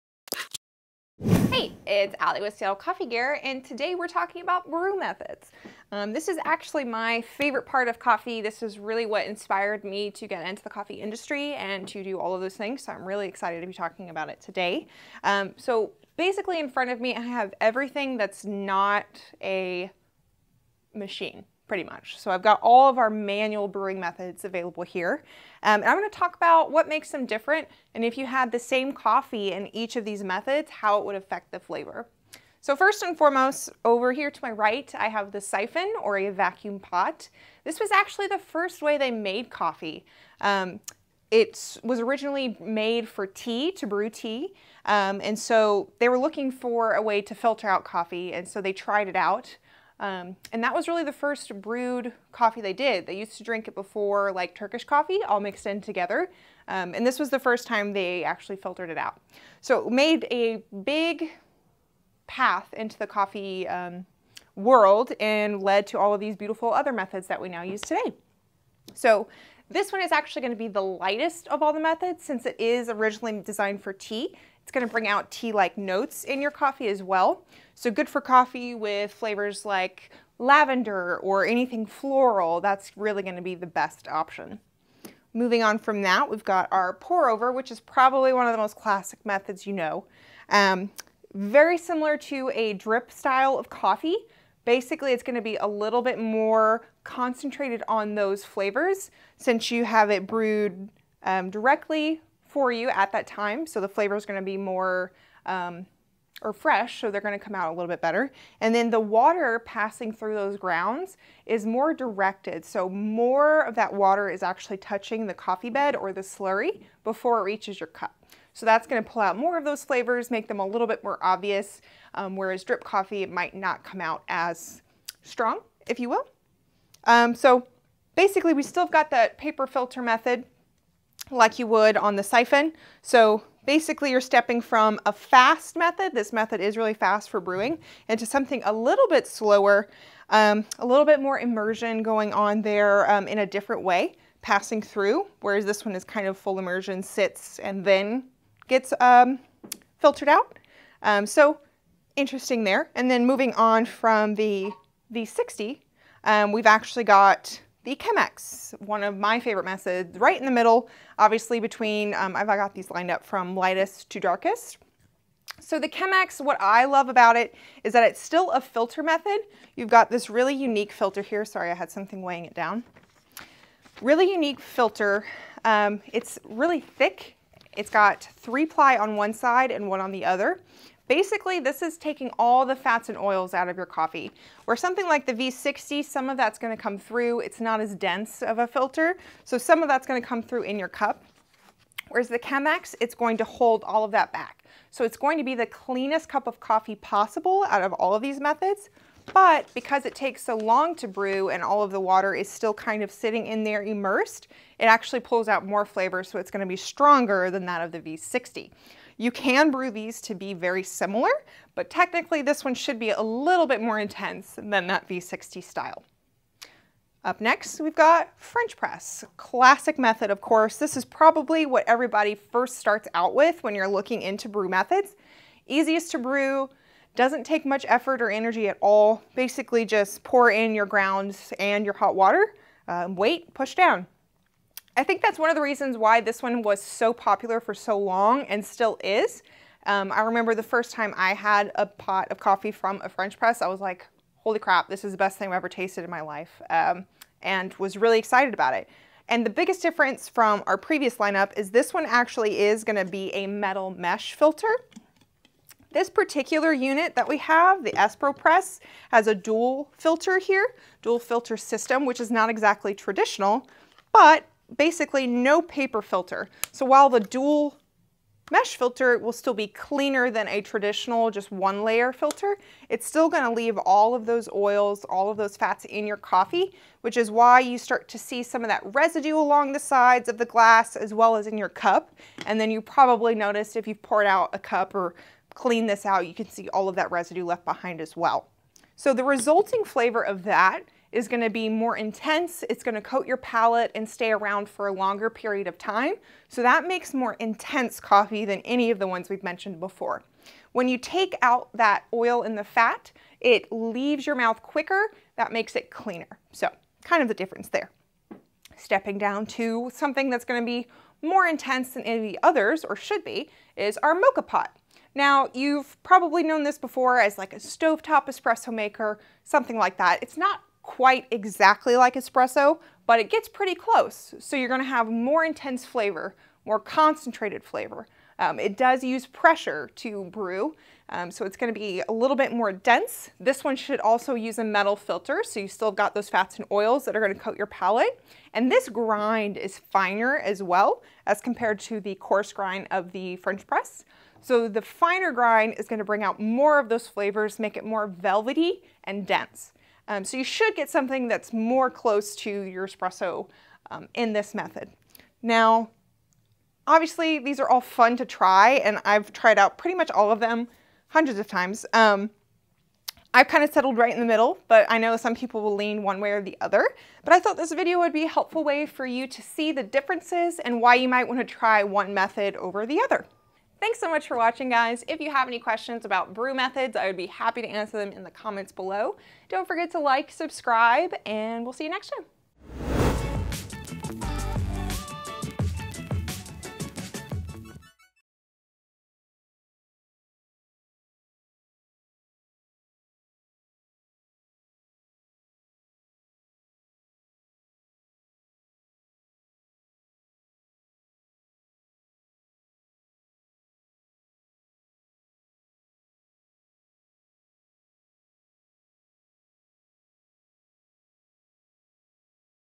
Different Ways to Brew With Allie
Learn about some different brew methods and the ways they affect the flavor of your coffee with Allie. This video features brew methods like stovetop brewing, pour over coffee, Chemex, and more.